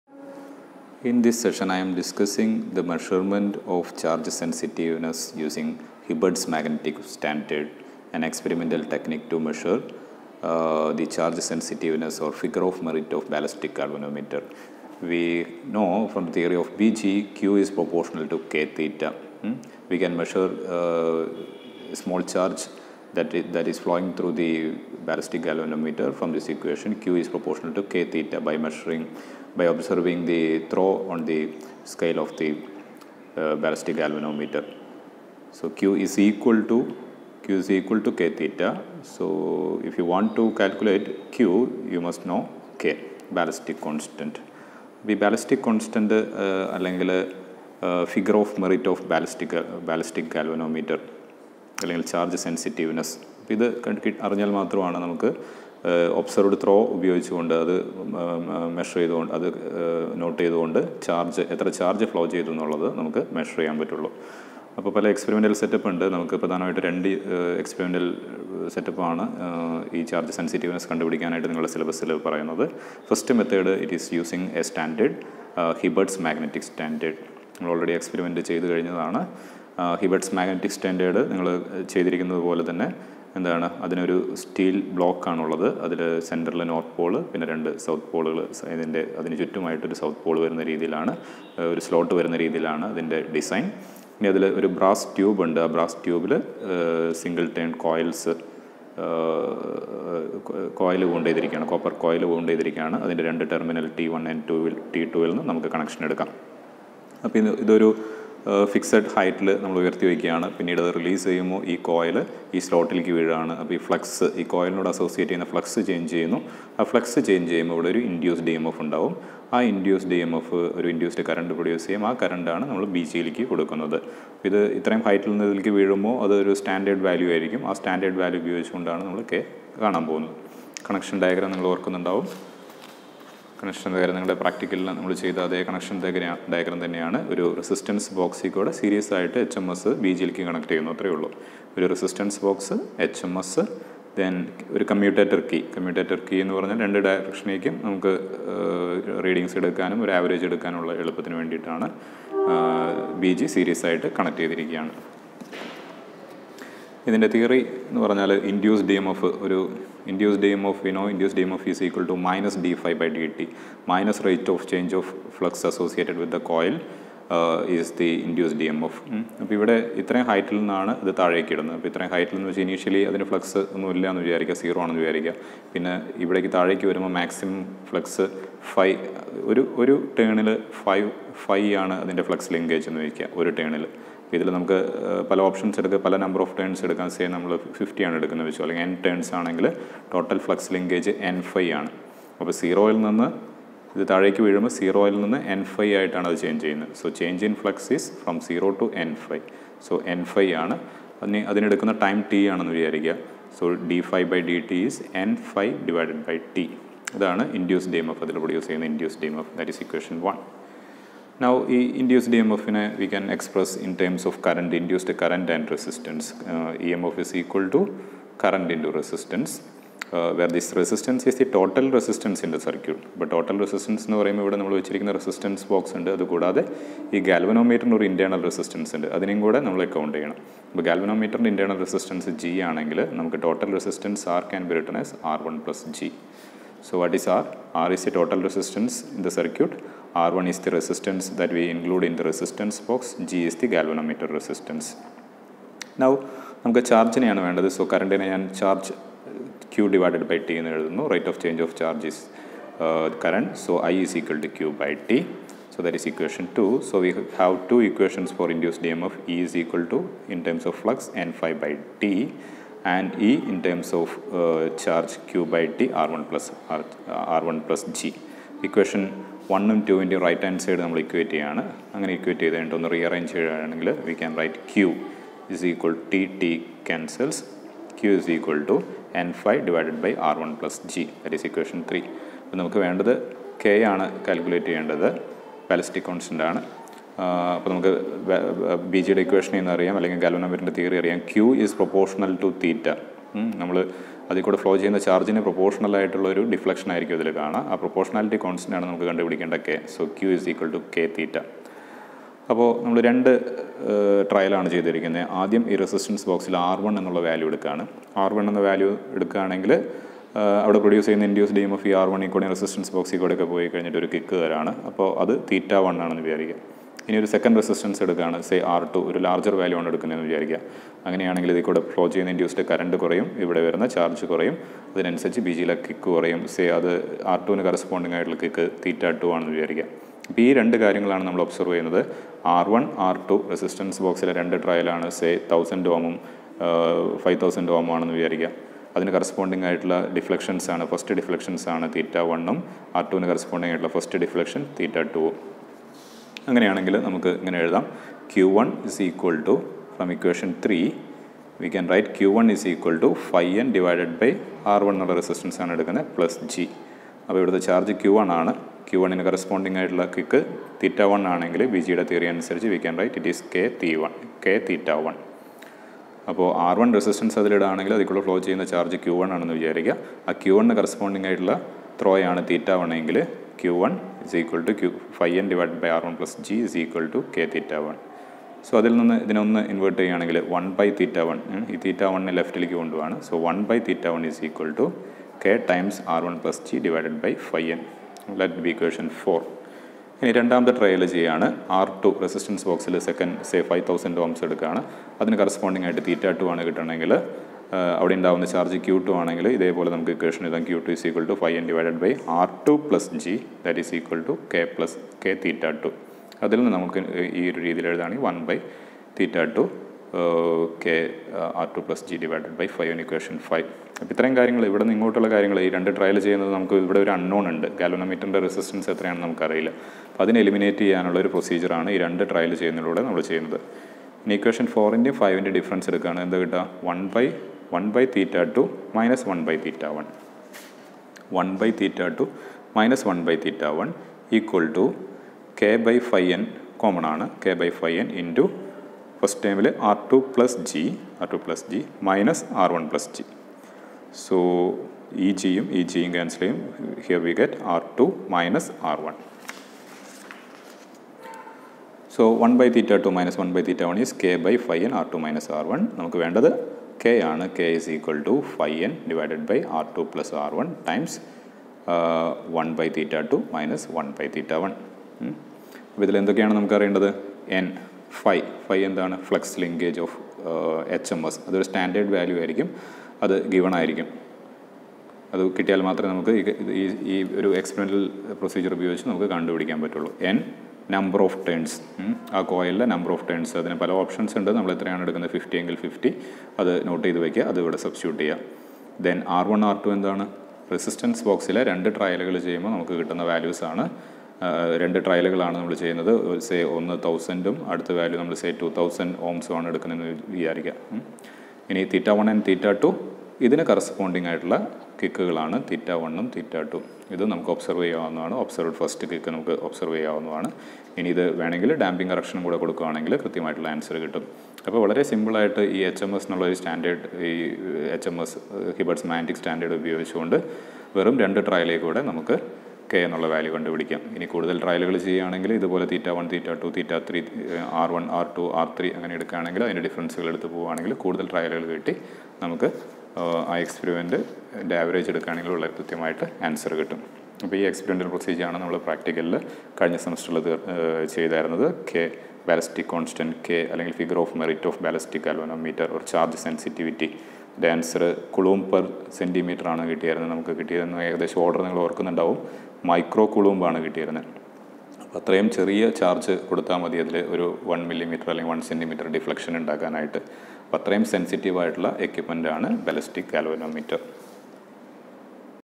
Charge Sensitiveness of BG using HMS
Measurement of Ballistic constant using Hibberts Magnetic Standard